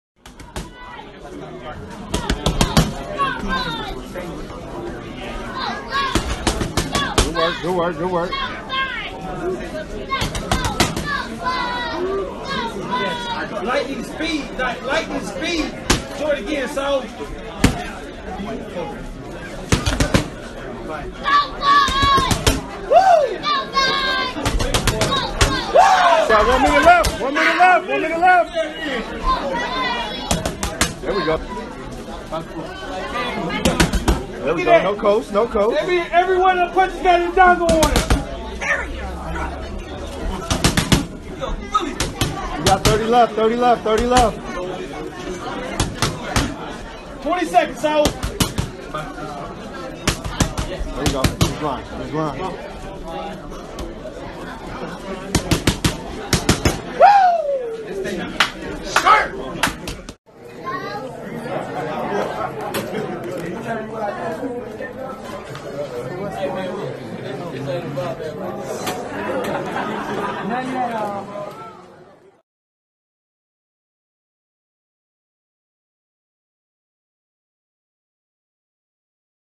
TERENCE CRAWFORD LIGHTS UP MITTS WITH QUICK COMBOS; LOOKS SMOOTH & READY TO SNATCH INDONGO'S STRAPS
Earlier today, WBC & WBO jr. welterweight champion Terence Crawford held his final media workout leading up to his highly-anticipated unification clash with WBA & IBF jr. welterweight champion Julius Indongo. Check out how he looked as he worked the mitts.